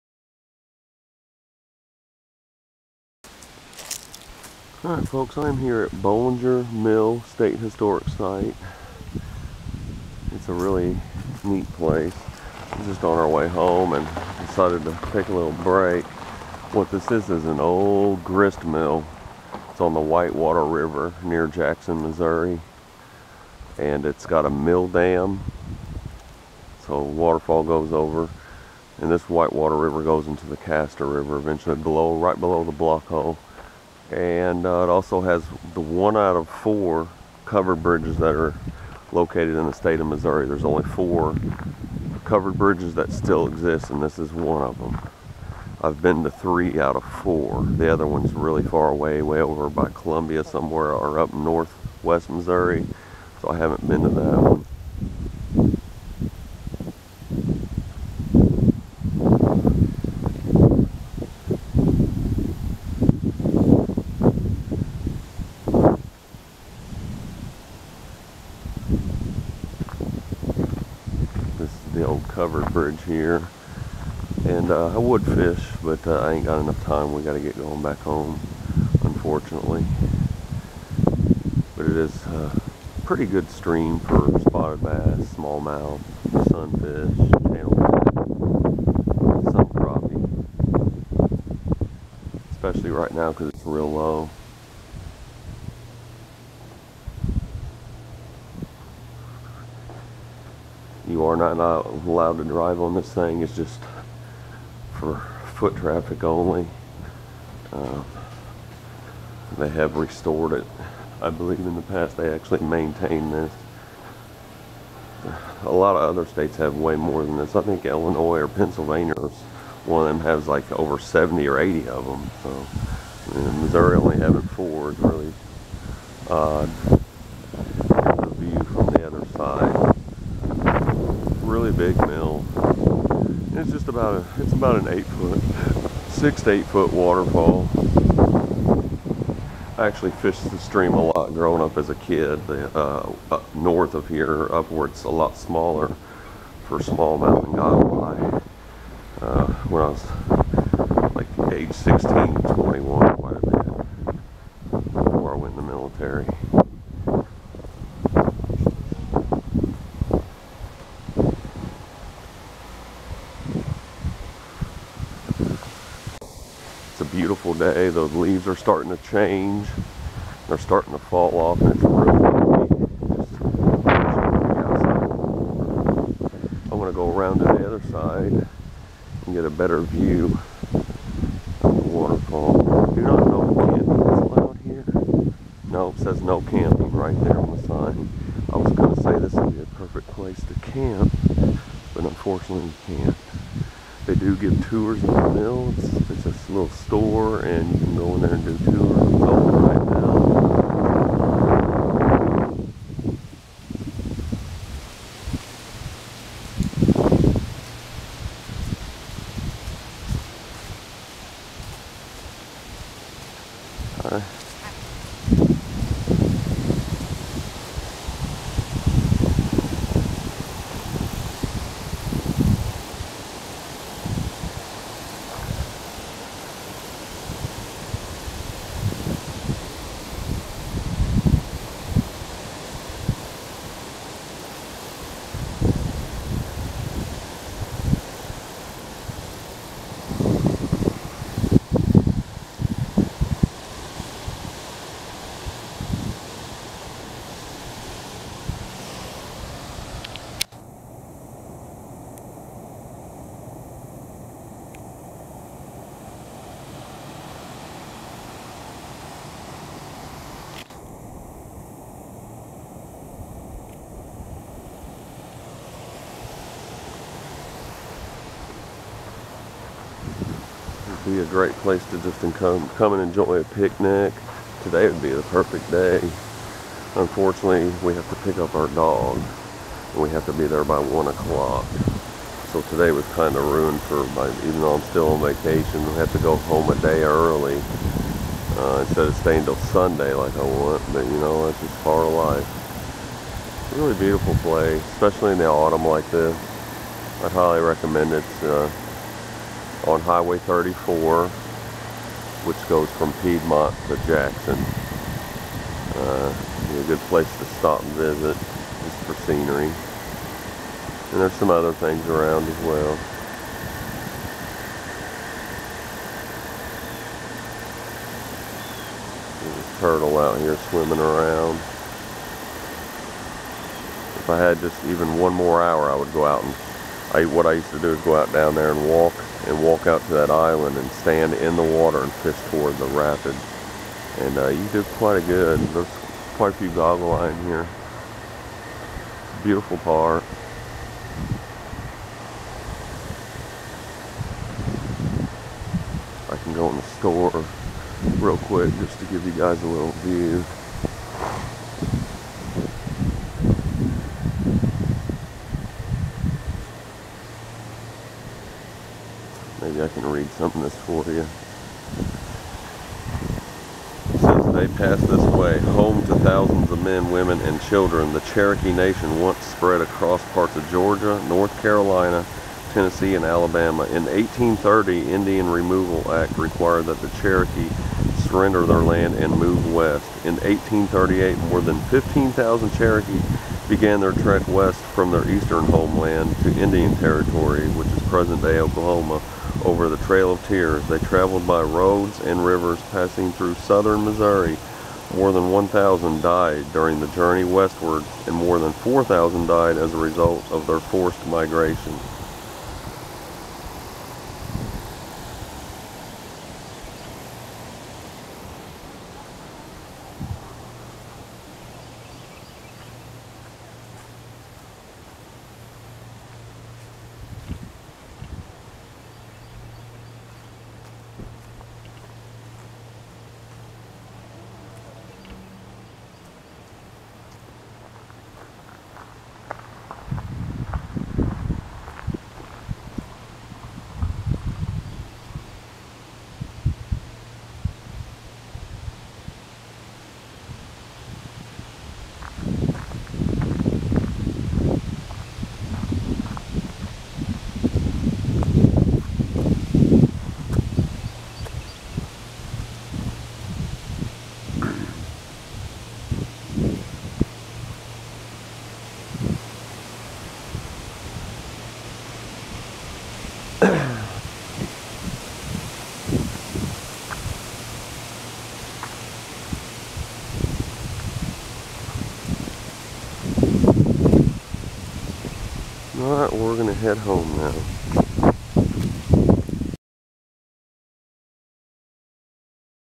Episode 108- Bollinger Mill Historic Site
This is where 1 of the the only 4 covered bridges exist in the State of Missouri, along with an old mill dam. Located on the Whitewater River, in the town of Burfordville, near Jackson, MO. in Bollinger County.